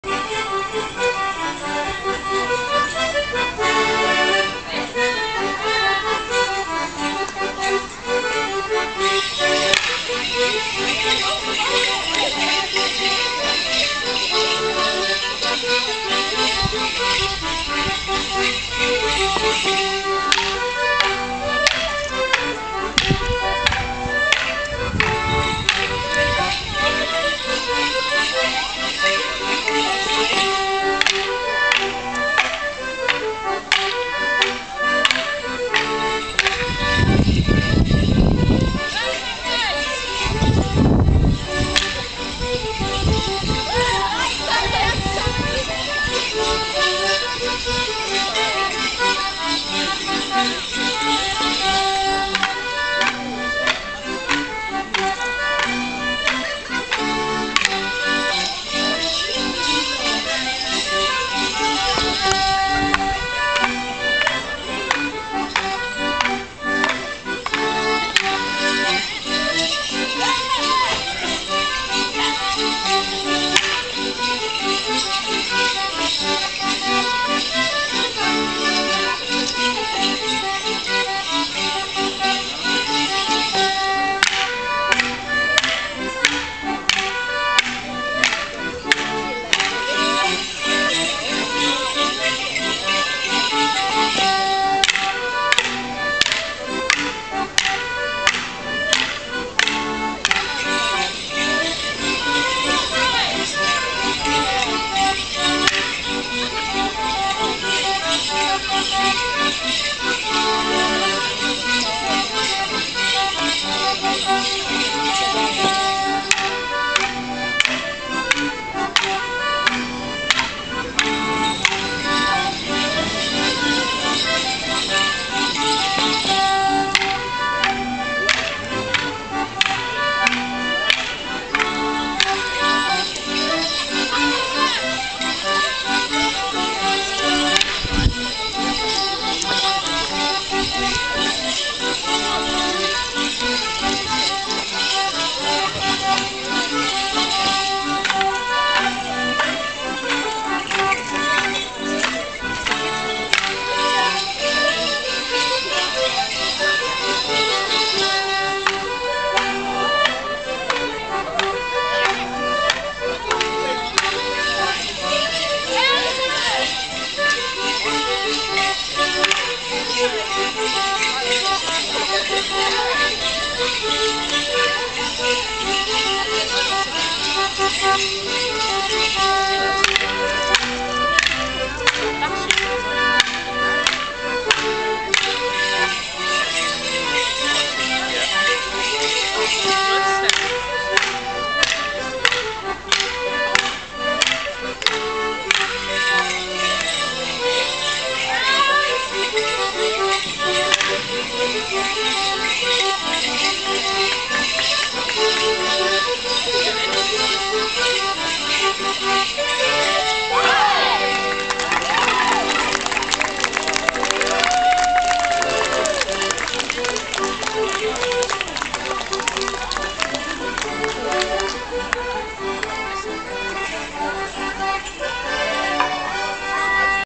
Earl of Stamford Morris - Sandbach Sept 3 2011
The Earl of Stamford Morris (younger members!) dance the Upton upon Severn Stick Dance at Sandbach Day of Dance, September 3rd 2011 outside the library.

Note that in this variant, everyone moves around the set one place at every given opportunity...!

Musicians: John Dover (English Concertina), Bernard Cromarty (Accordion).

The other side seen in this clip (not dancing) are Persephone.